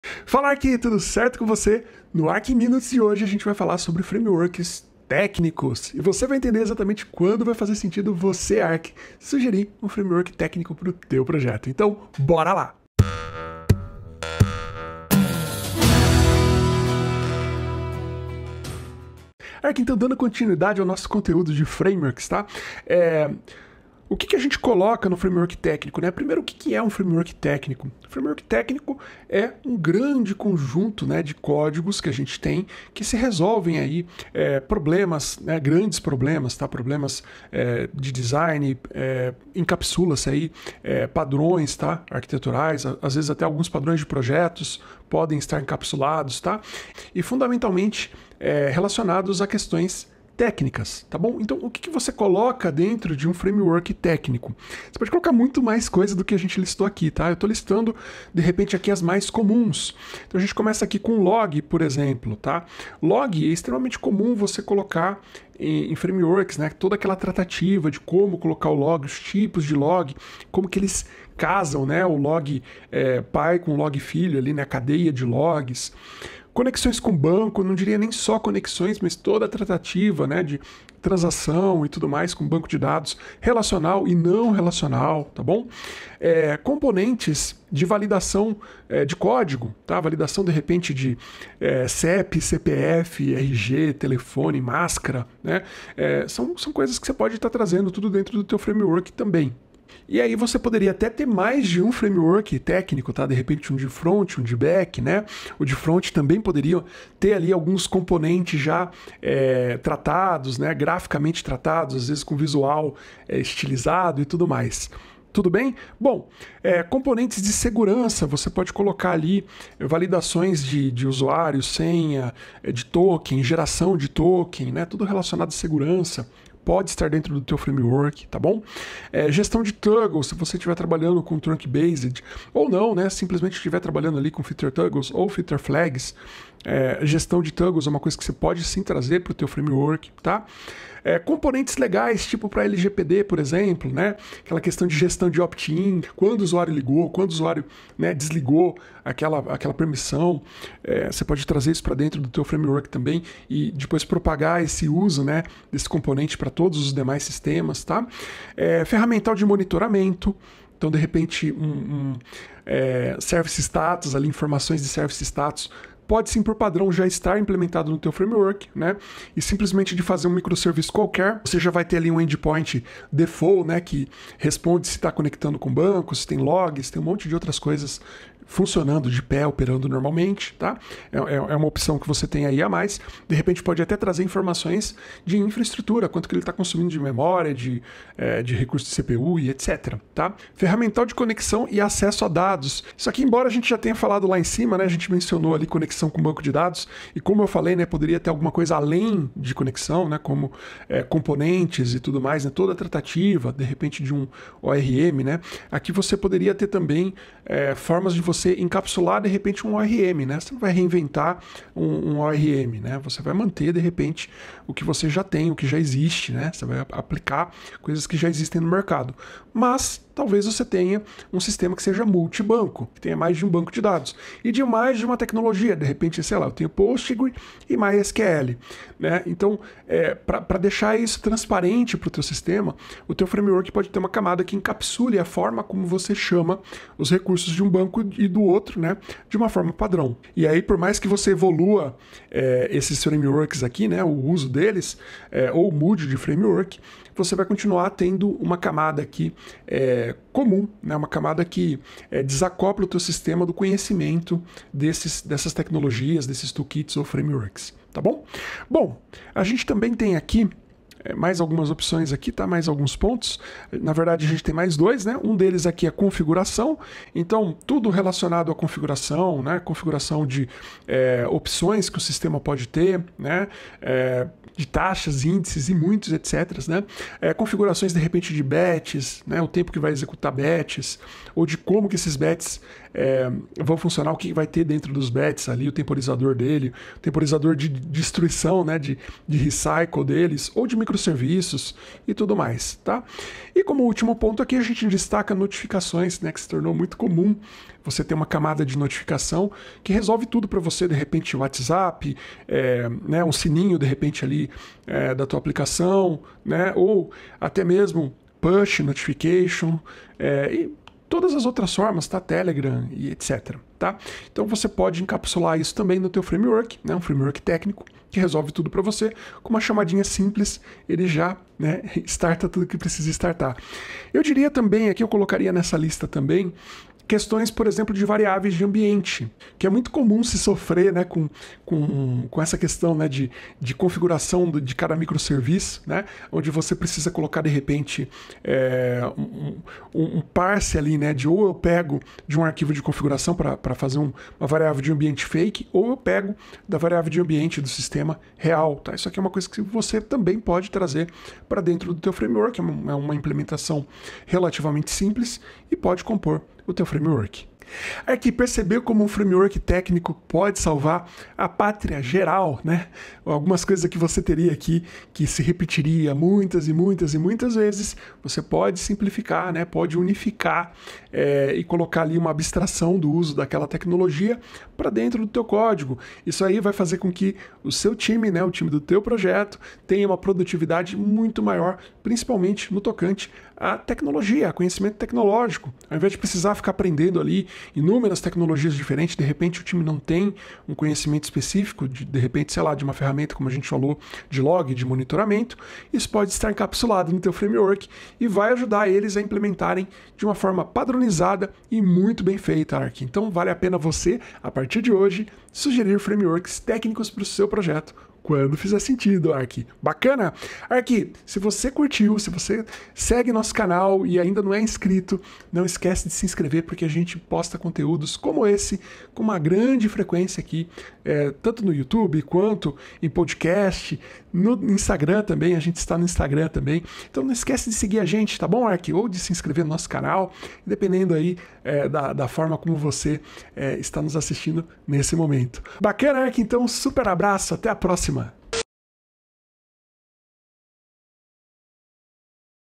Quando sugerir um FRAMEWORK TÉCNICO?
Um framework técnico, é um conjunto de ferramentas, bibliotecas, padrões e diretrizes que são usados para desenvolver soluções em uma determinada área de tecnologia. Ele fornece uma estrutura para os desenvolvedores trabalharem e ajuda a acelerar o processo de desenvolvimento de software, simplificando o desenvolvimento de aplicativos complexos.

Geralmente, um framework técnico inclui componentes reutilizáveis ​​que podem ser personalizados e combinados para criar soluções específicas. Esses componentes abrangem áreas como interface do usuário, gerenciamento de dados, segurança, gerenciamento de sessões, autenticação, entre outros.

Ao usar um framework técnico, os desenvolvedores podem se concentrar na lógica de negócios e nas necessidades específicas do aplicativo, em vez de gastar tempo criando código básico para funções comuns. Isso pode aumentar a produtividade, melhorar a qualidade do código e acelerar o tempo de lançamento do produto final.

Depois deste conteúdo você certamente vai considerar implementar no seu projeto.
Embora atualmente seja pouco direcionado no mercado, FRAMEWORKS TÉCNICOS podem fazer toda a diferença em um trabalho de arquitetura de software, tornando o desenvolvimento incrivelmente mais padronizado, organizado e focado nas reais necessidades dos projetos.